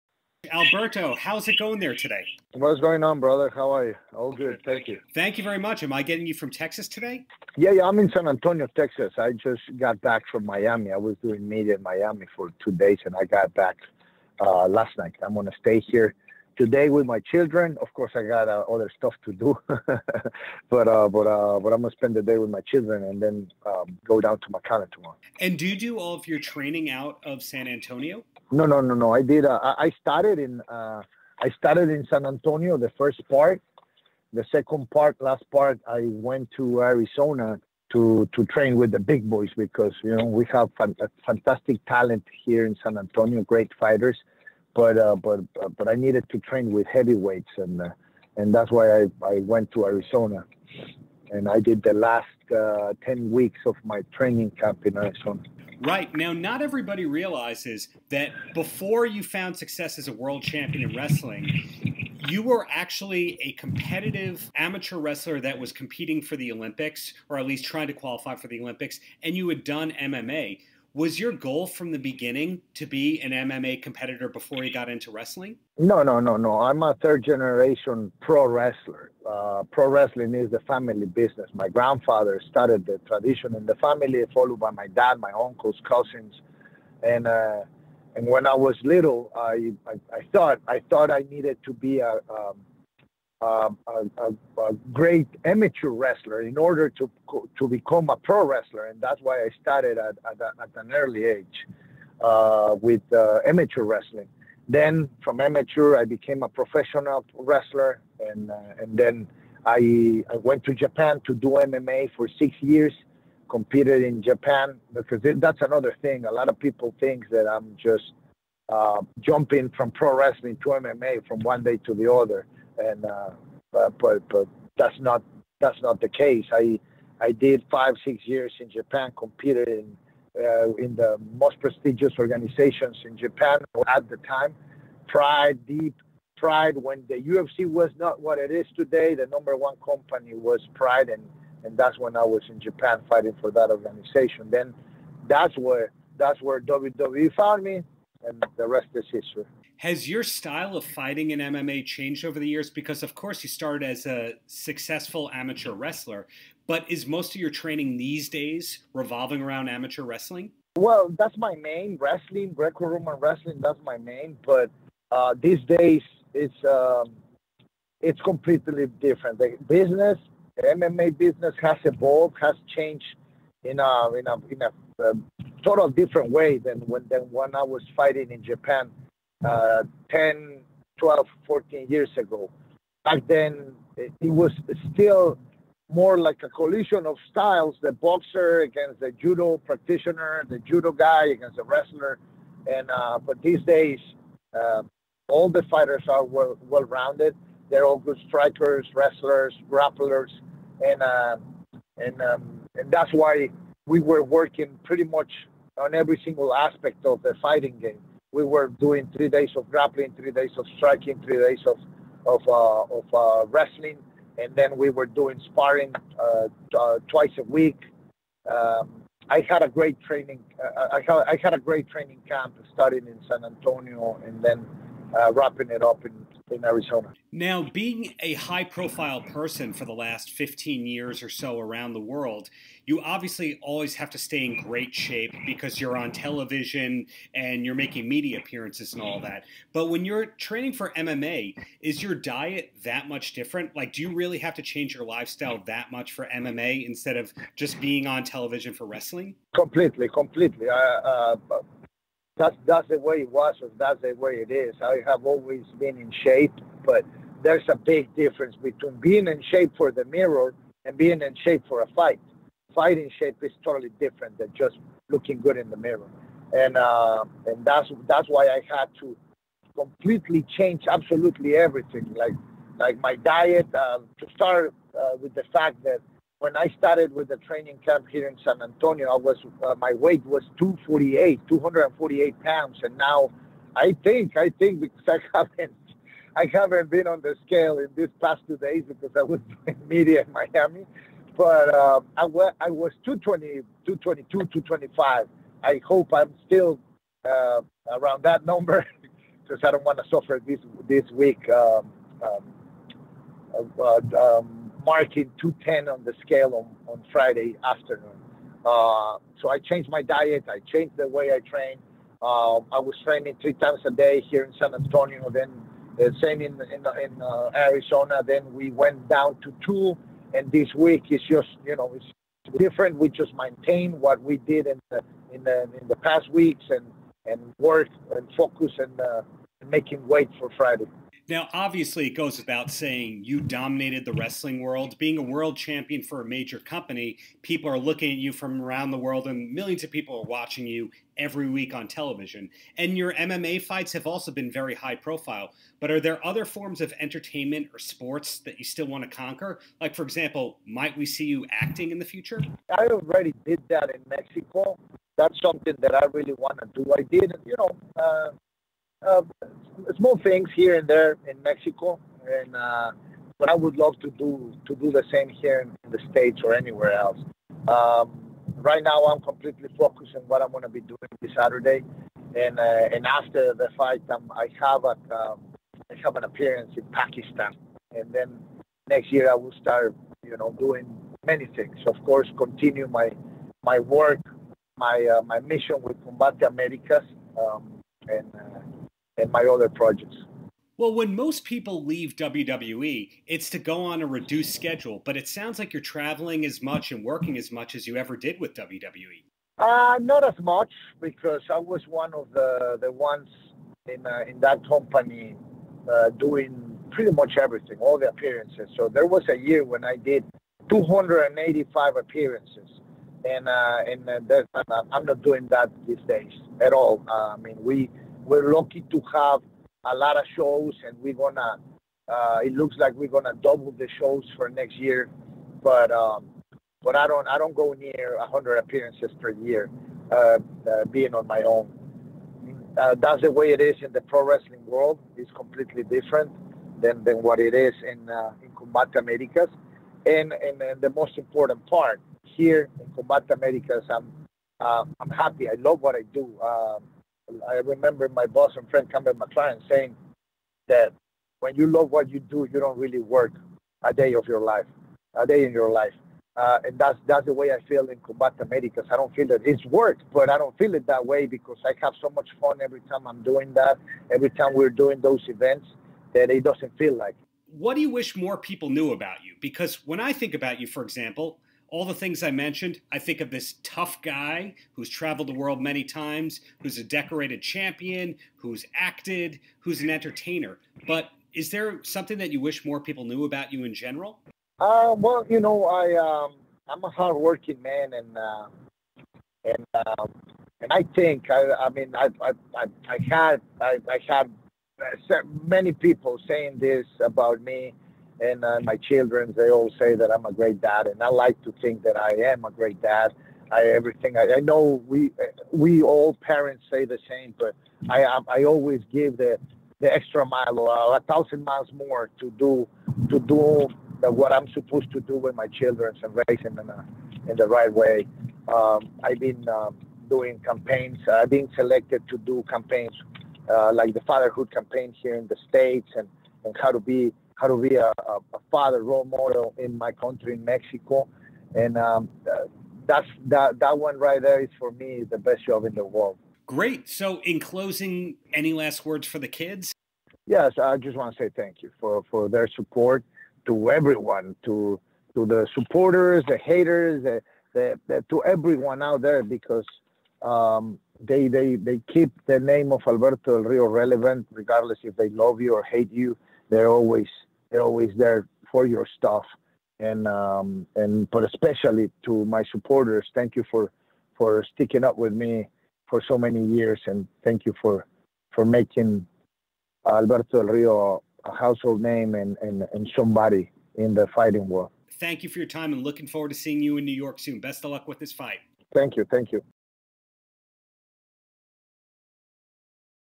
Alberto Del Rio interview with Darren Paltrowitz
Former WWE World Champion and current MMA star Alberto Del Rio spoke with Darren Paltrowitz (host of the "Paltrocast With Darren Paltrowitz") about his upcoming fight with Tito Ortiz for "Combate Americas 51," training for MMA versus professional wrestling, upcoming career plans and more. Recorded December 3, 2019.